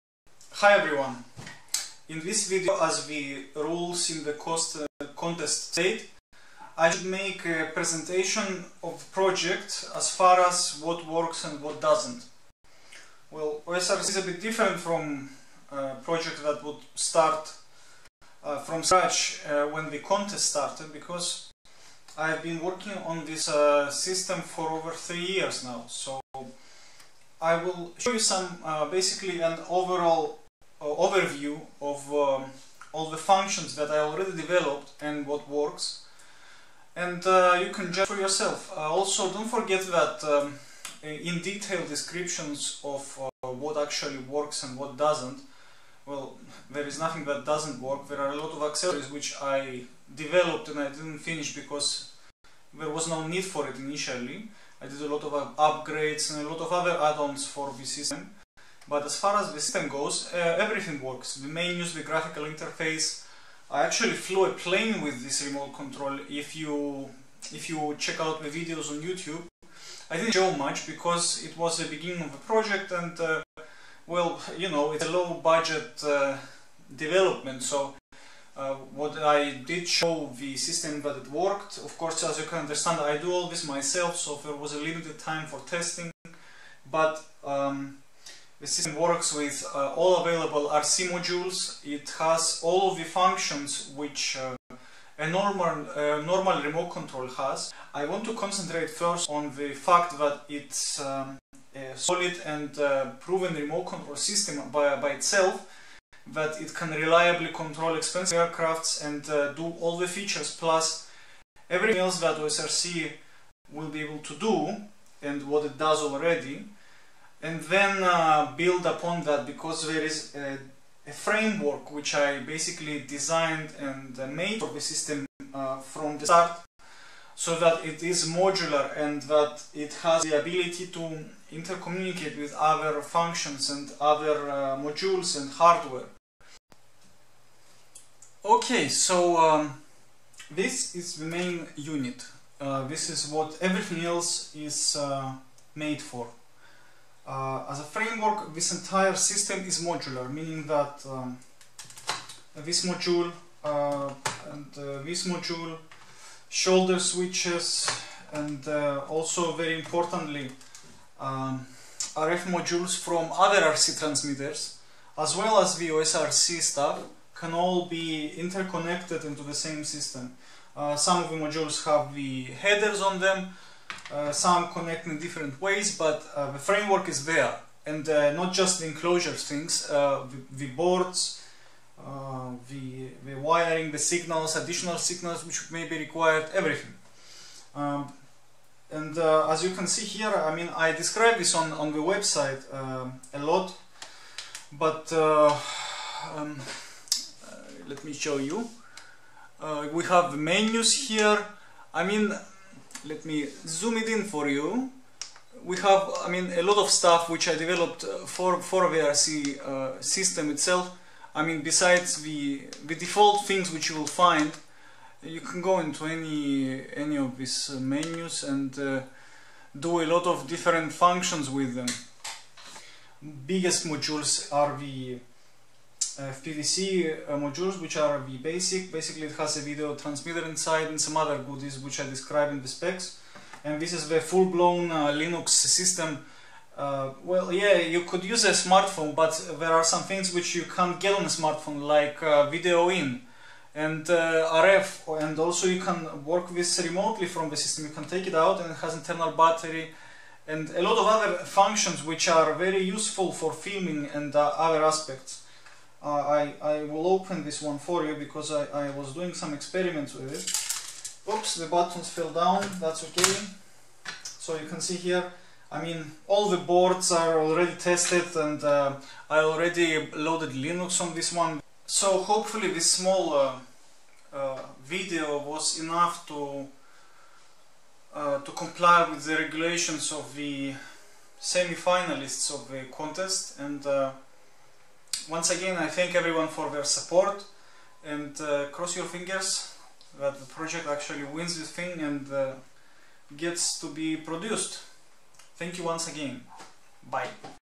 Hackaday What Works What Doesn't
This is a short video made for the Hack A Day Prize contest describing what works and what doesn't for to the judges. Naturally, those who have been following the project for a while now, know that the system is far beyond the prototype stages so this is more of a typicality for the contest than anything else but still should be interesting to most.